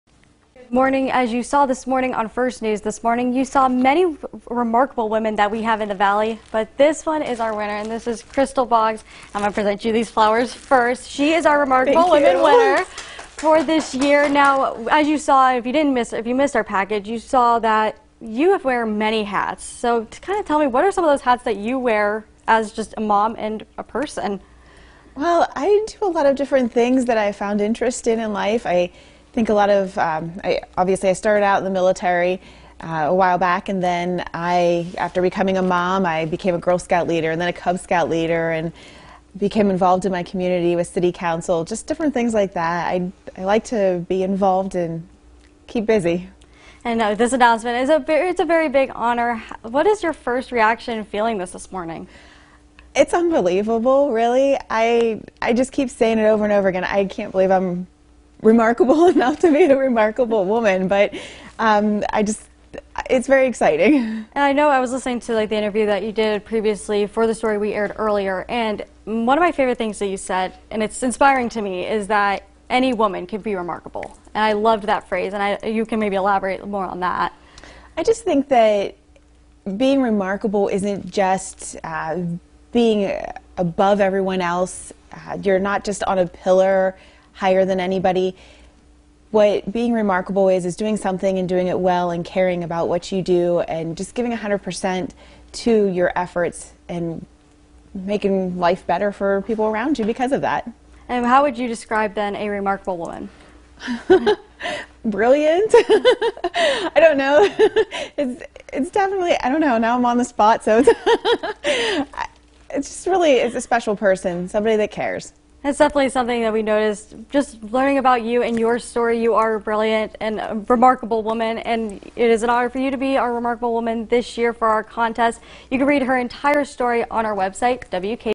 WKBN introduces this year's remarkable woman winner
Monday, they introduced the winner, Crystal Siembida Boggs of the Siembida Boggs Philanthropic Foundation.